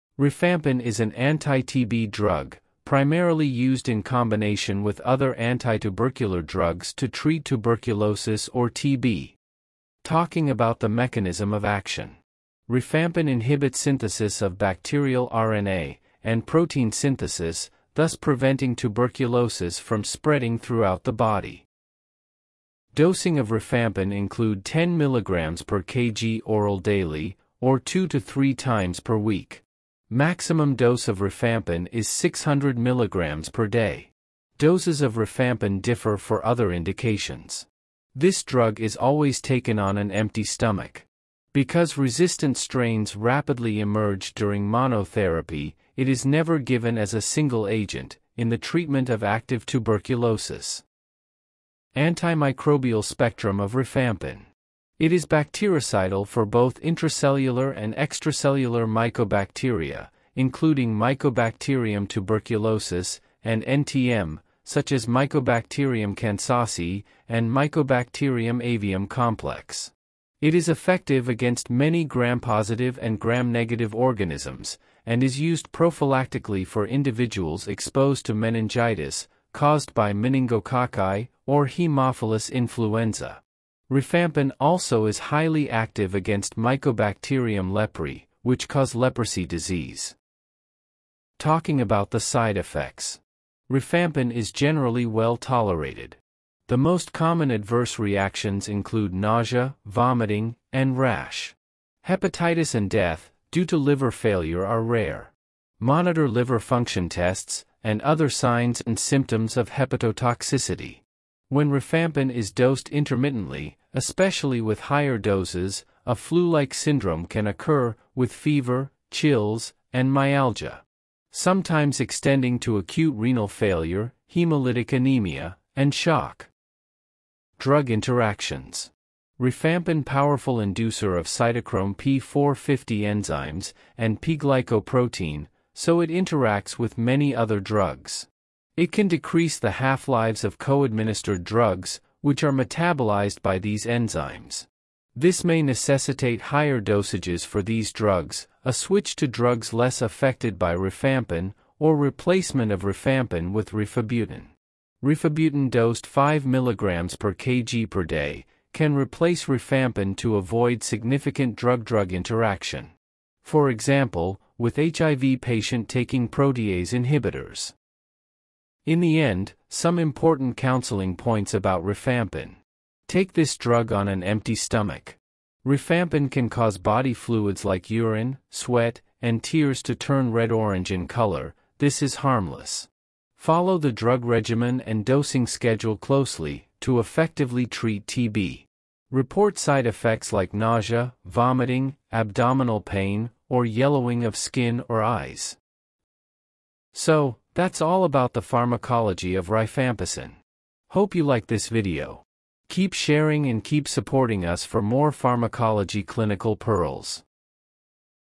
Rifampin (anti TB drugs) pharmacology, antitubercular drugs pharmacology, pharmacology made easy
rifampin, antibiotics, antimycobacterial, antitubercular drugs pharmacology, antibiotics pharmacology, anti TB drugs pharmacology, pharmacology made easy,

💊 Rifampin is a bactericidal antibiotic from the rifamycin class primarily used to treat tuberculosis and leprosy. It works by inhibiting bacterial RNA polymerase, thereby blocking RNA synthesis and preventing bacterial replication.

💊 Rifampin is well-absorbed orally and metabolized by the liver, inducing cytochrome P450 enzymes, which can lead to significant drug interactions. Common side effects include hepatotoxicity, gastrointestinal disturbances, and orange discoloration of bodily fluids. It is typically used in combination therapy to prevent resistance.

Chapters:
0:00 - rifampin pharmacology (antimicrobial drugs)
0:10 - rifampin mechanism of action
0:22 - rifampin daily dose
0:51 - rifampin spectrum of activity
1:30 - rifampin side effects
2:07 - rifampin drug interactions
2:48 - rifampin clinical pearls